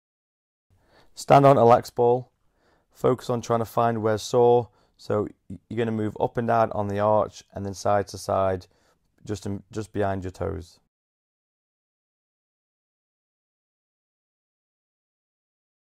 Sole Smash
Stand on a hard ball and roll around